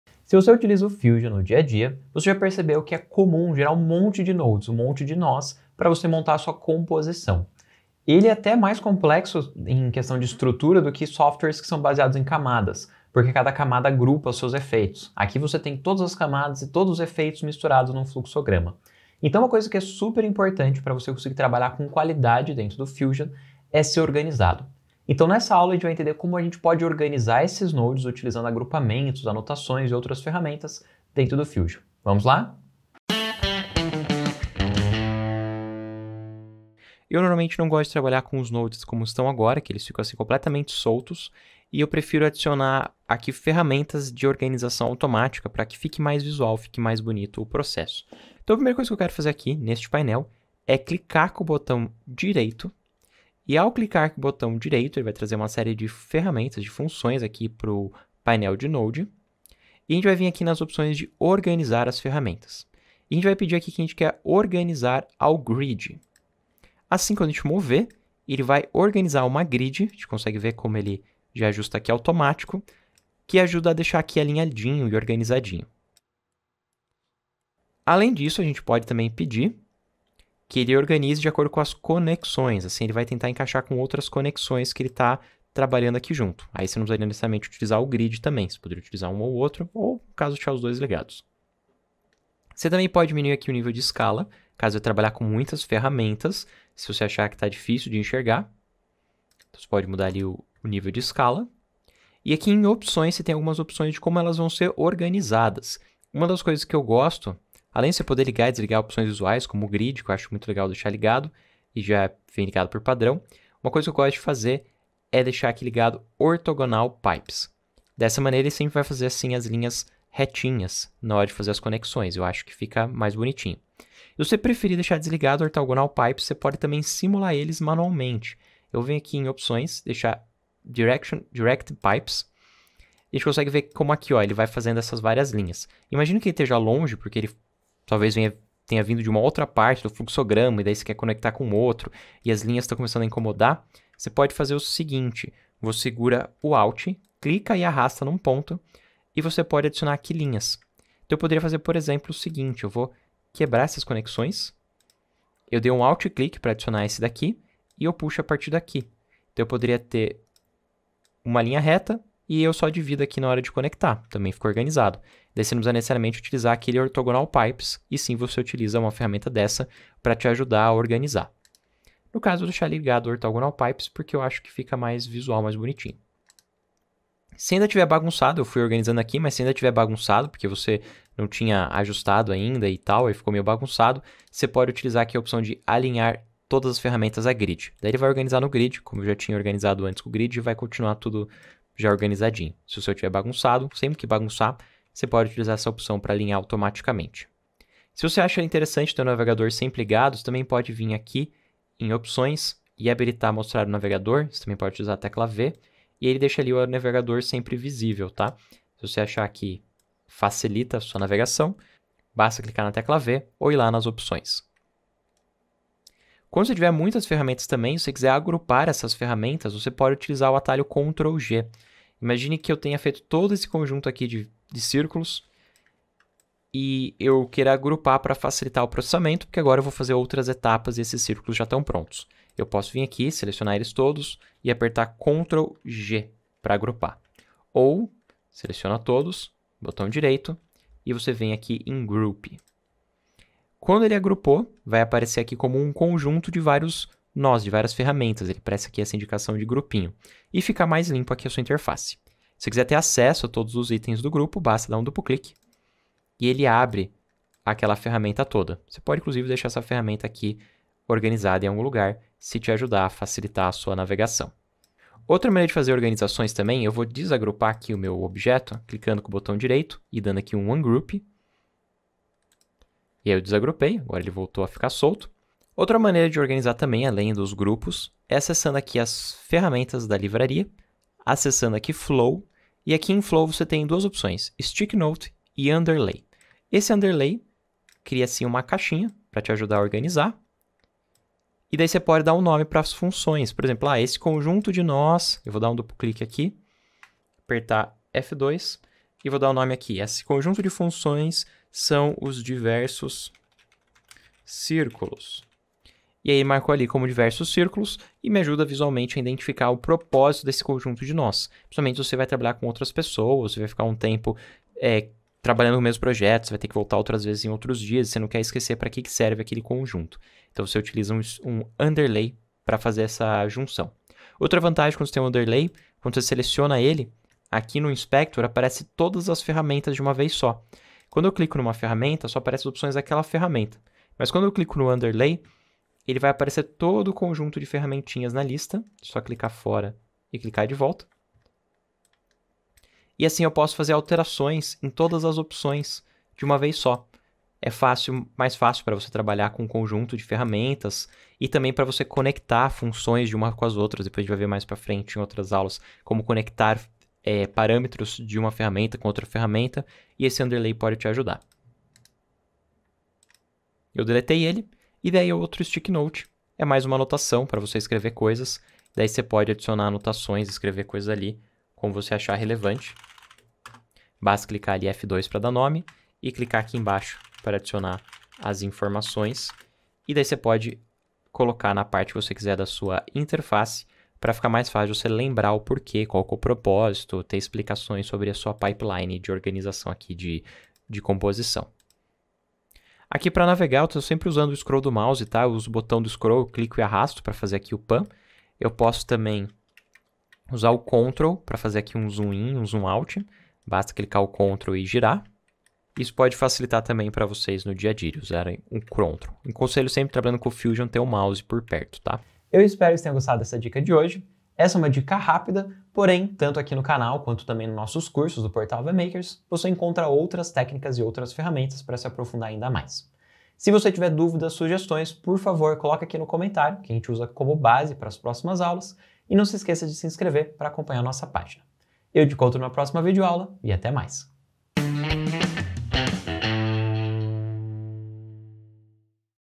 DaVinci Fusion Básico: Organização e Agrupamento de Nodes
O Fusion, da Blackmagic, agora também está presente dentro do DaVinci Resolve e pode ser utilizado para realização de efeitos visuais e motion graphics diretamente da linha de edição!

Nessa aula você irá aprender a organizar e agrupar os Nodes, para que seu projeto não acabe virando uma bagunça com a quantidade de nodes que você irá trabalhar.

Vamos lá?!

Grande abraço!!